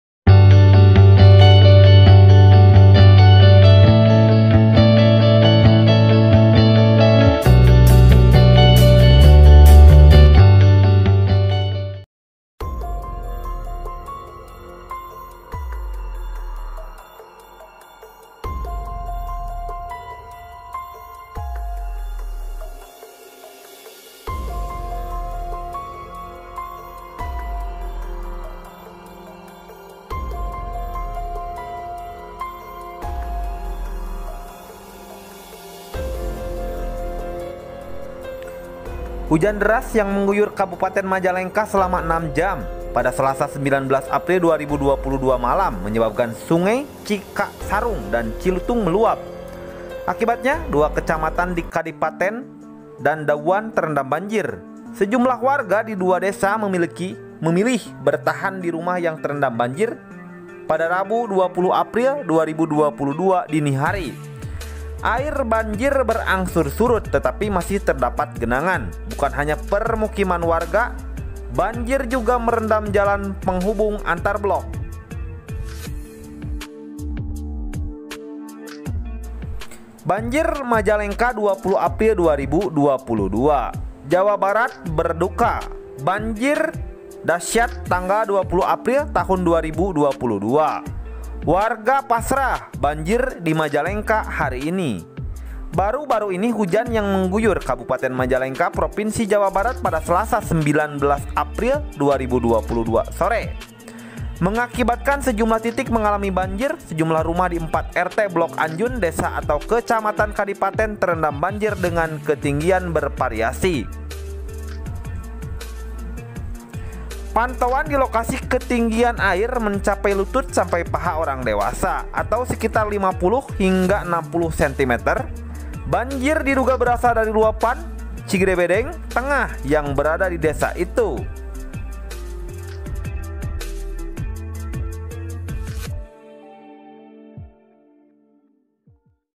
Majalengka Kaget!! Banjir Hebat Sapu Majalengka Hari Ini, Banjir Majalengka Hari Ini
Jawa barat Berduka! Banjir mengamuk 20 April 2022, warga panik, banjir di Majalengka Hari Ini.

baru baru ini hujan deras yang mengguyur kabupaten Majalengka Jawa barat pada Selasa 19 April 2022 sore, mengakibatkan sejumlah rumah di 4 RT blok anjun, desa atau kecamatan kadipaten Terendam banjir dengan ketinggian bervariasi.